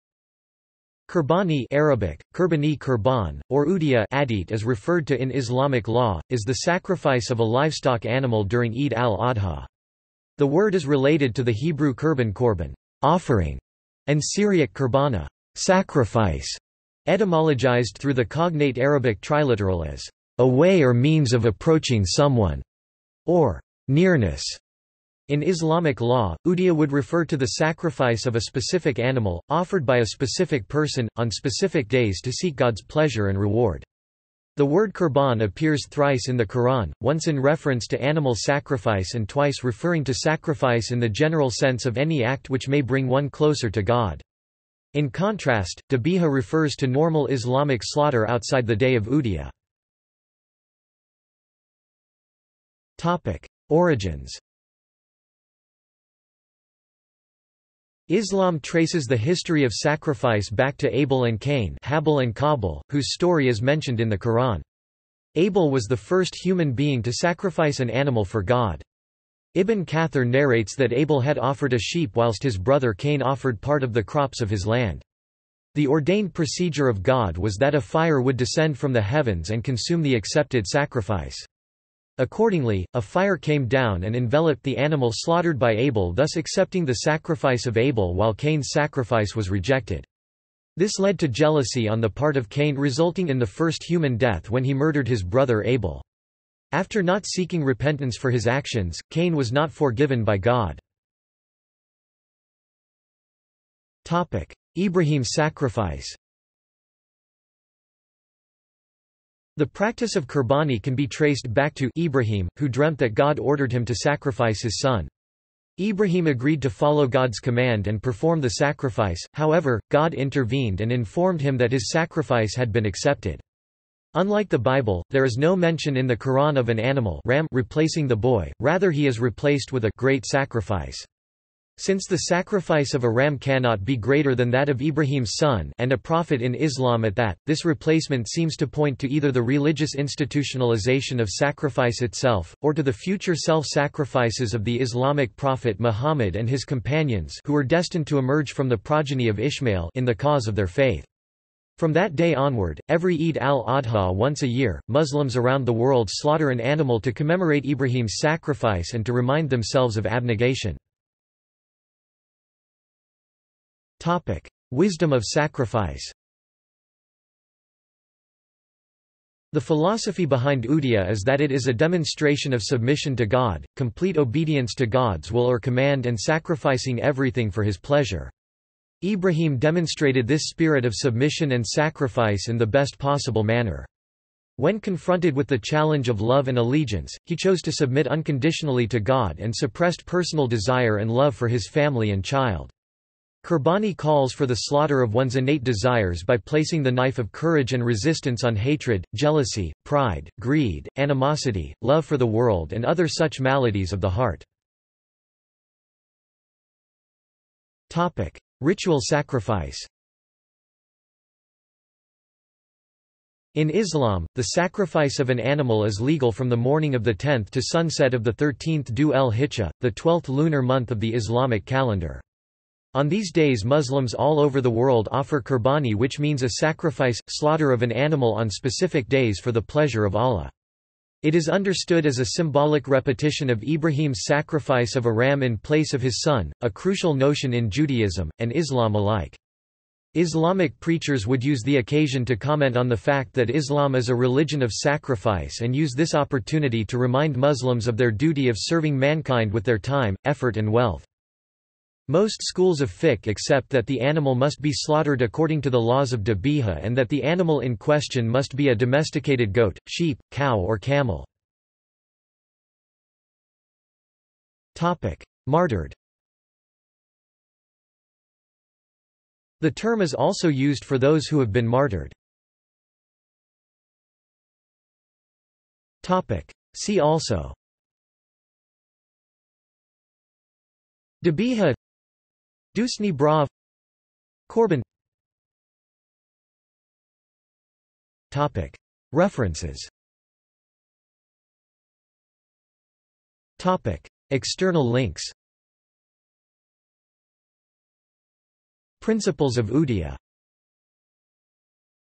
Qurbani | Wikipedia audio article
This is an audio version of the Wikipedia Article:
Qurbani

- Socrates

SUMMARY
Qurbanī (Arabic: قربانى‎), Qurban, or uḍḥiyyah (أضحية) as referred to in Islamic law, is the sacrifice of a livestock animal during Eid al-Adha. The word is related to the Hebrew קרבן‬ qorbān "offering" and Syriac qurbānā "sacrifice", etymologised through the cognate Arabic triliteral as "a way or means of approaching someone" or "nearness". In Islamic law, udhiyyah would refer to the sacrifice of a specific animal, offered by a specific person, on specific days to seek God's pleasure and reward. The word qurban appears thrice in the Quran: once in reference to animal sacrifice and twice referring to sacrifice in the general sense of any act which may bring one closer to God. In contrast, dhabīḥah refers to normal Islamic slaughter outside the day of udhiyyah.